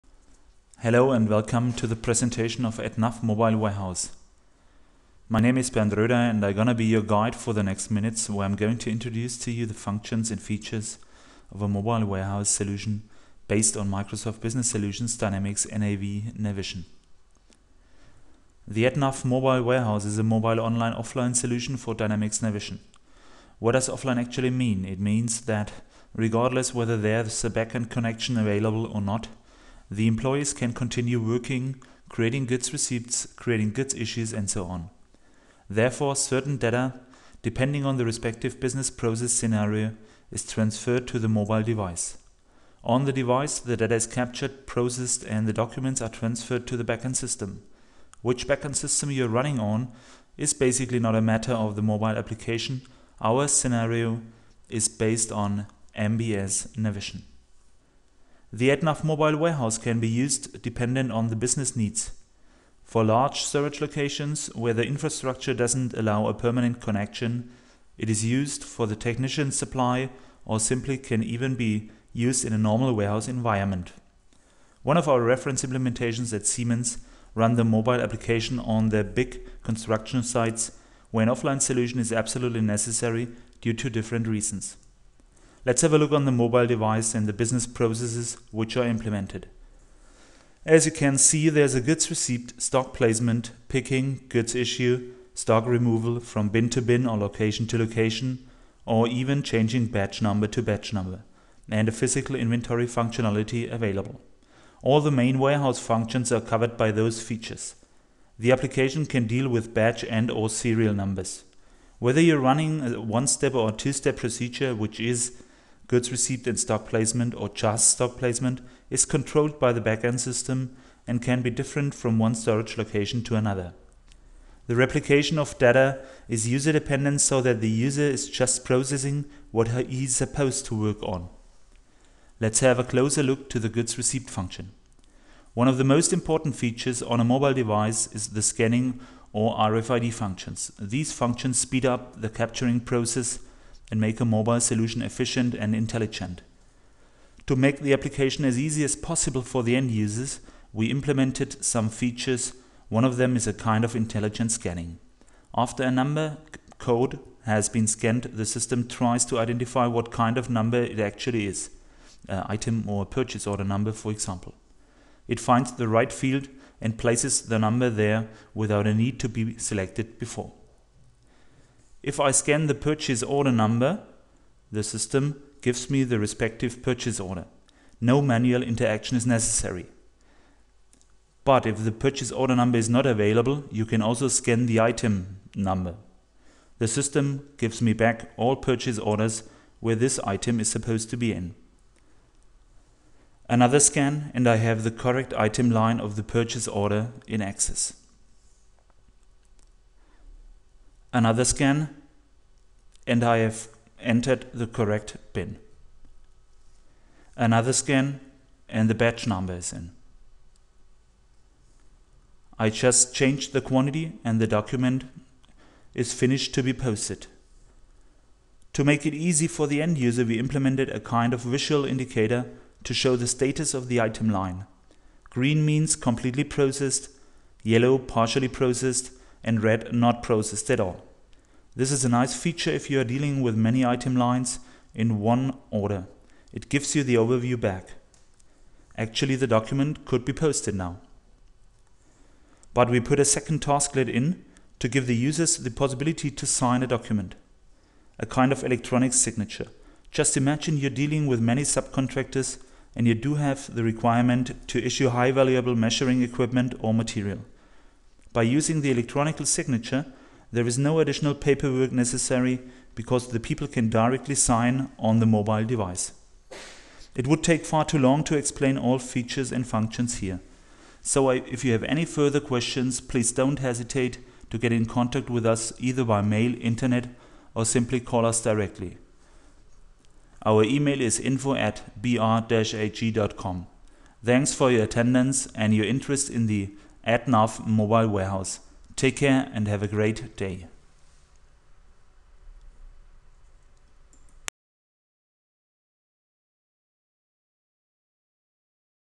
Mobile Warehouse \ Mobile WM Solution for Navsion
Video presentation of a mobile Offline/Online Warehouse Management solution based on MBS Dynamics NAV / former Navision. Batch/Serial No. enhanced, BarCode scanning functions, RFID, Mobility, Dynamics Mobile.
Solution by bartolome roeder AG, Munich Germany.